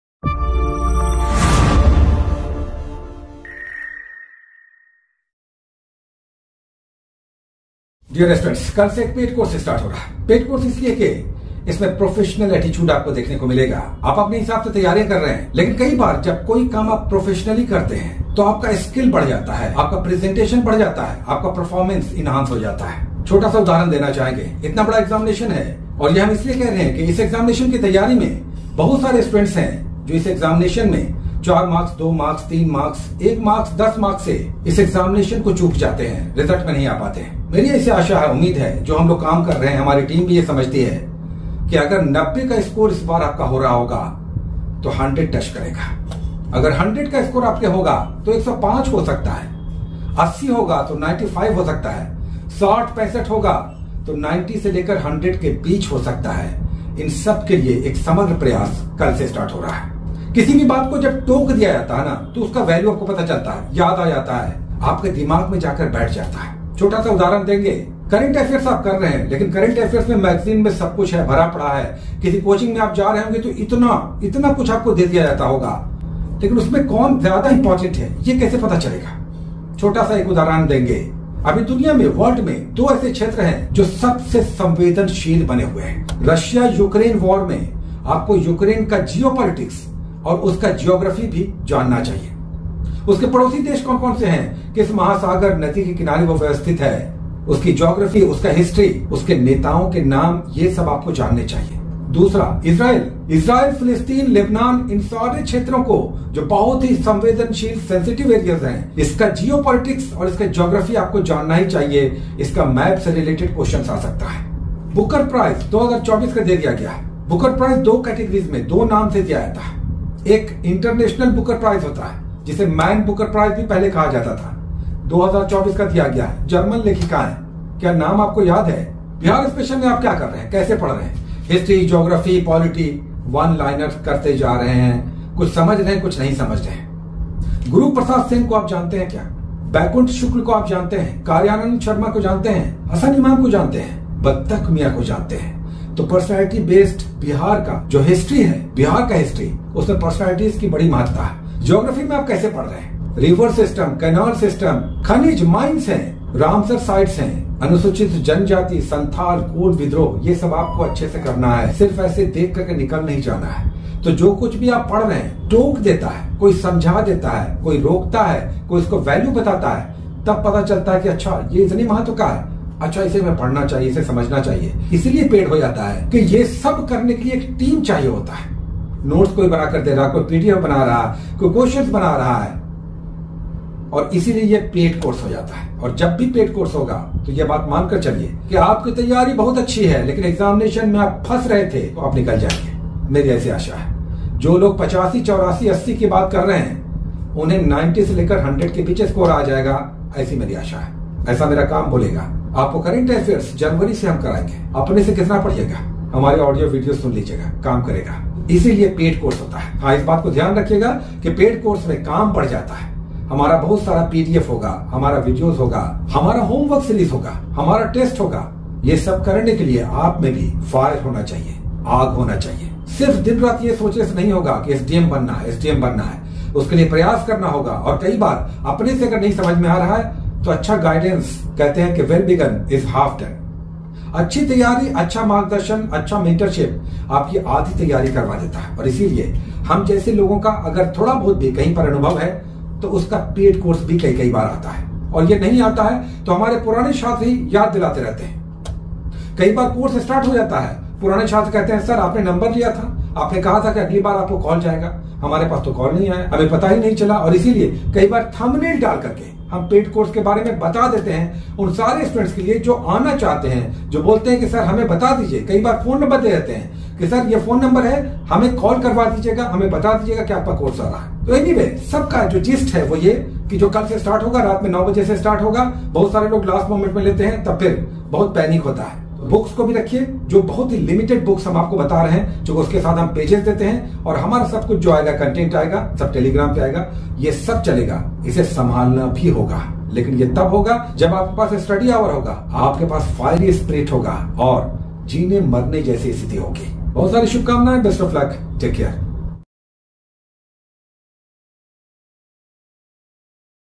70th BPSC || 50 Din ... Paid Course , Kal Se Start !!
70th BPSC || 50 Din ... Paid Course , Kal Se Start !!
Dear Students !
%0 din ka paid course kal se start ho rahaa hai . Jo bhi aapne padha , jo aap padhh rahe hain , usme 10 se 40 marks ka difference aayega , is course ke baad.
Video mein humne bataa diya hai ki kya aapko fark dikhega.Aap kewal padhh rahe hain kya nahi padhna hai , wo bhi padhh rahe hain . Isi samsya ko ye course door karega !